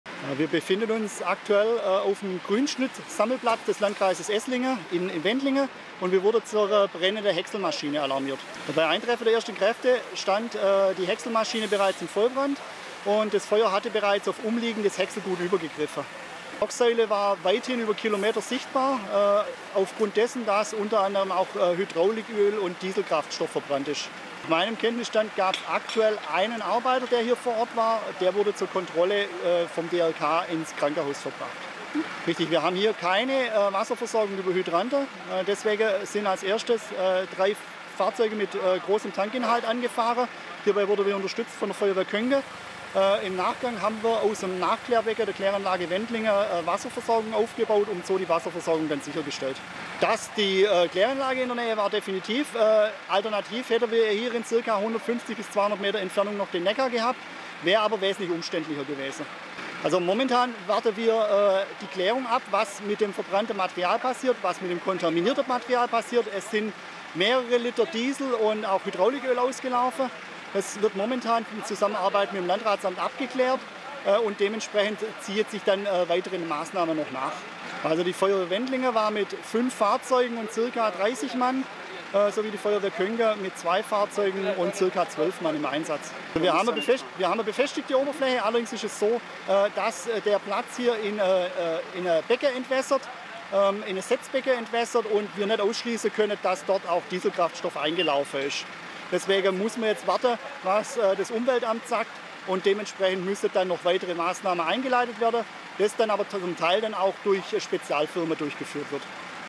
23.09.2022 - Siebmaschine in Brand geraten
Wendlingen (pol) - Zum Brand einer Siebmaschine in einer Kompostierungsanlage sind Feuerwehr, Rettungsdienst und Polizei am Freitagmittag ausgerückt. Kurz vor zwölf Uhr hatte der Motor der Maschine aus noch unbekannter Ursache zu rauchen begonnen, kurze Zeit später stand das Gerät in Flammen. Die Rauchsäule war kilometerweit zu sehen. Die Feuerwehr, die mit fünf Fahrzeugen und 25 Einsatzkräften angerückt war, löschte die Flammen. Auch der Rettungsdienst war vorsorglich angerückt. Verletzt wurde nach derzeitigem Kenntnisstand niemand. Der entstandene Sachschaden dürfte sich auf mehrere zehntausend Euro belaufen.